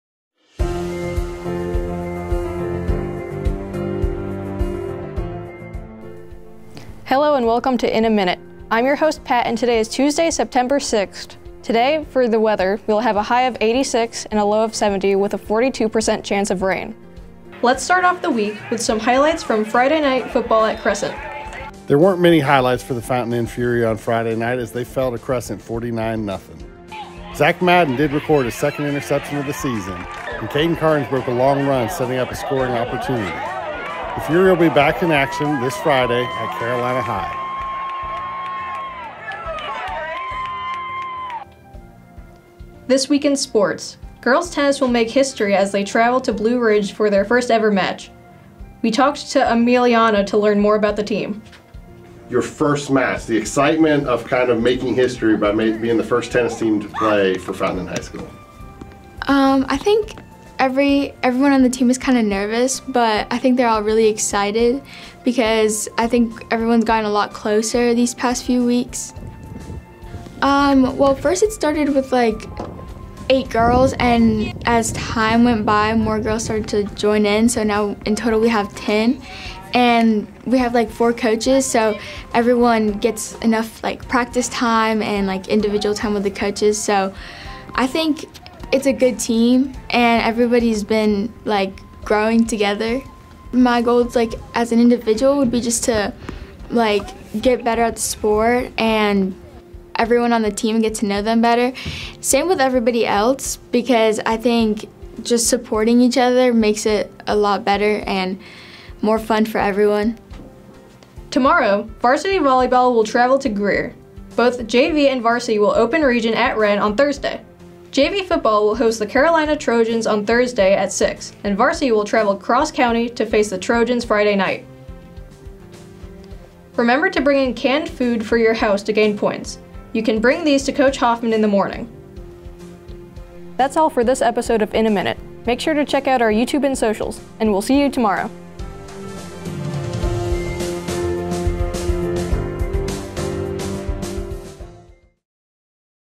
INN a Minute (Yr 2 Ep 12) September 6, 2022 - 1st Girls Tennis Match Preview
Coming off the long weekend, we are excited to interview a member of our Girls Tennis team as they prepare to play in their historic first match tonight at Blue Ridge. Pat also brings us updates on football's trip to Crescent and a look ahead at "This Week INN Sports".